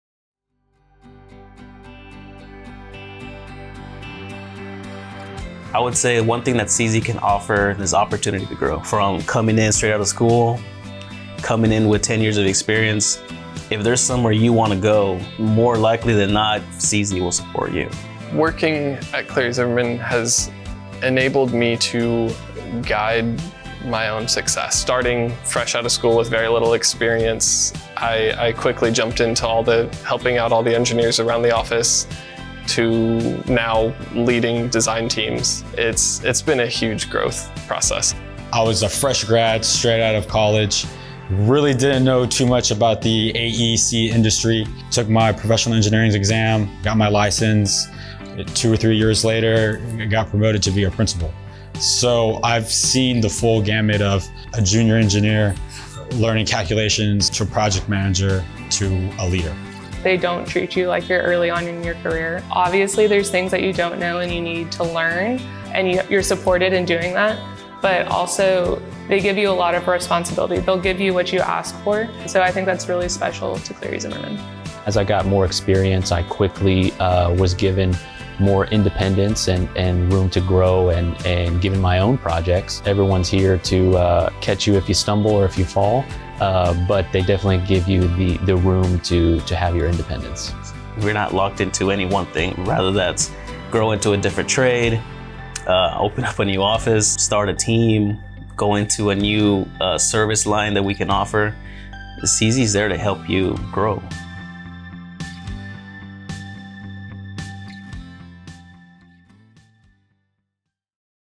Growth at Cleary Zimmermann
"Working at Cleary Zimmermann has allowed me to guide my own success." Opportunities for growth abound at Cleary Zimmermann! Hear from engineers who have experienced  this growth firsthand.